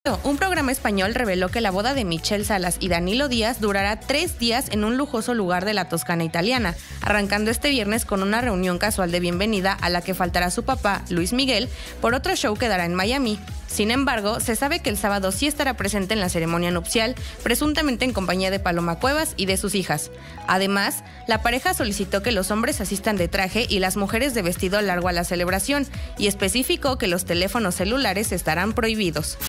Michelle Salas prohíbe usar celulares en su boda
La boda de Michelle Salas durará 3 días en Toscana, región de Italia. Aseguran que prohibió el uso celular, además los hombres deberán vestir de traje y mujeres con vestido largo.

Grupo Fórmula

Canal sugerido

Descarga nuestra